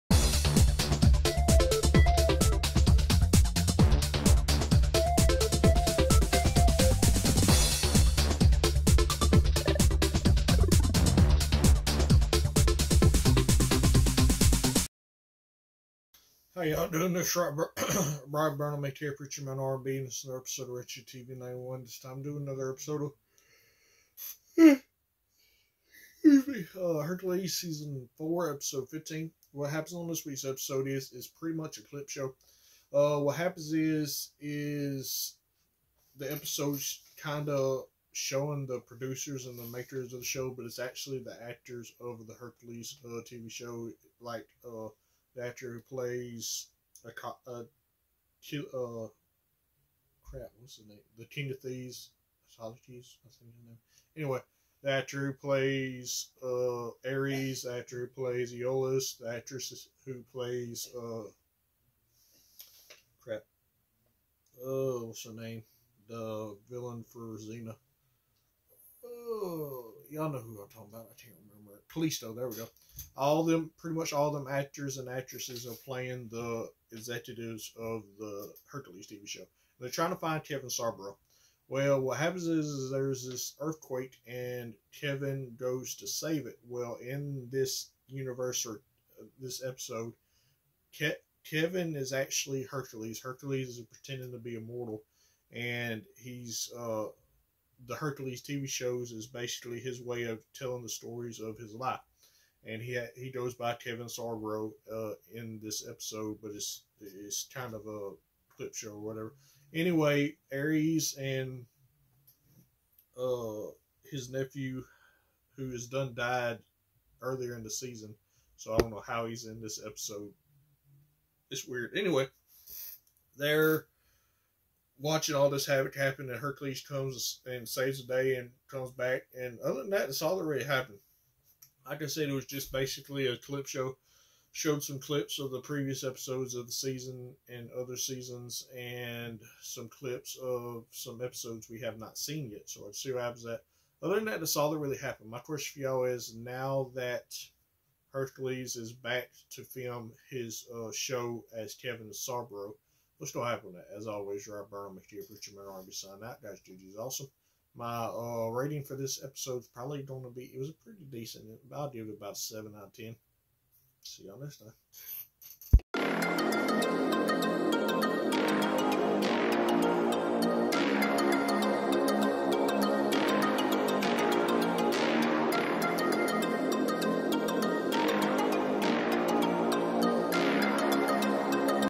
Hercules reviews season 4 ep. 15.
This is another episode of Hercules reviews and this time we are reviewing season 4 episode 15.